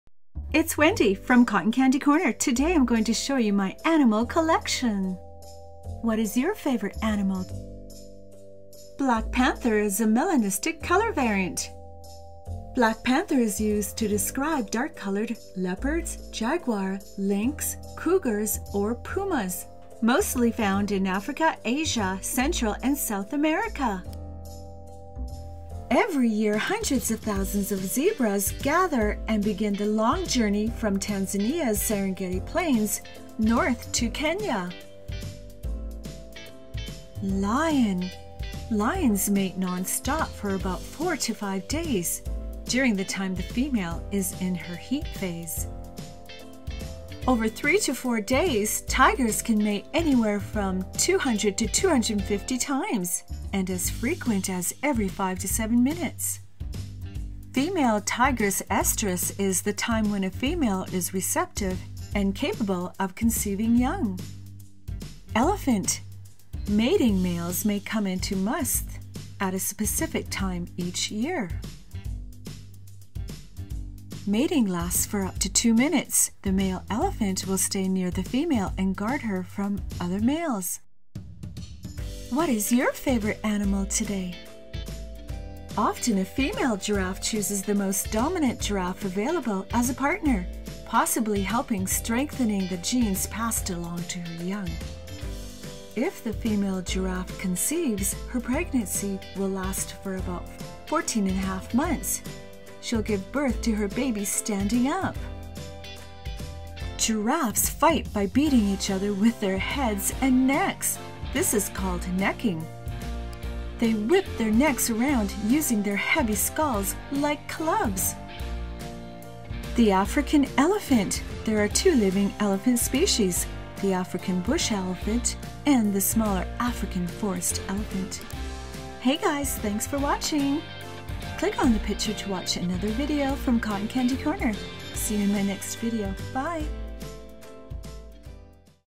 Collection of Big Cats and Zoo Animals - Lion Tiger Elephant Zebra Giraffe
Musth or must is a periodic condition in bull elephants characterized by highly aggressive behavior and accompanied by a large rise in reproductive hormones. Testosterone levels in an elephant in musth can be on average 60 times greater than in the same elephant at other times.